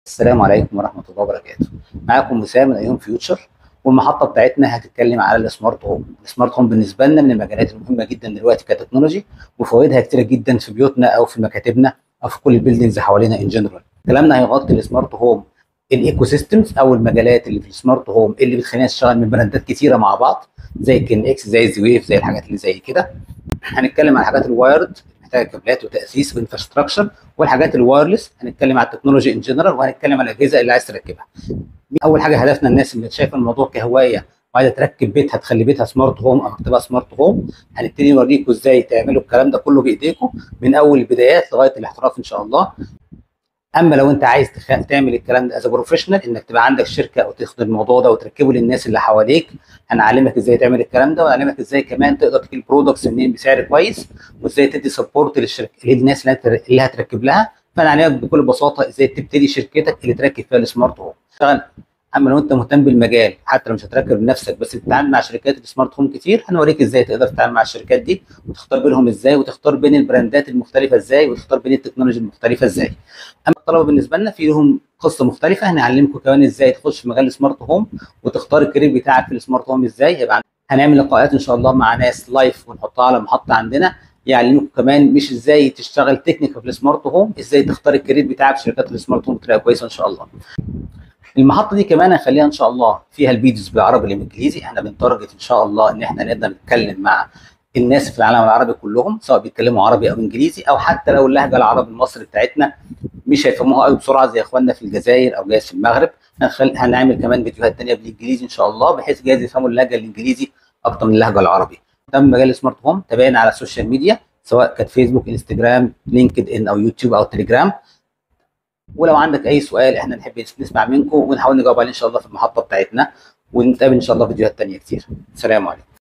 iHomeFuture's Youtube Channel Promotion and Goals
This is our vision in our channel, to help everyone who is interested somehow in smart home technology, to build a community with the knowledge and proper experience.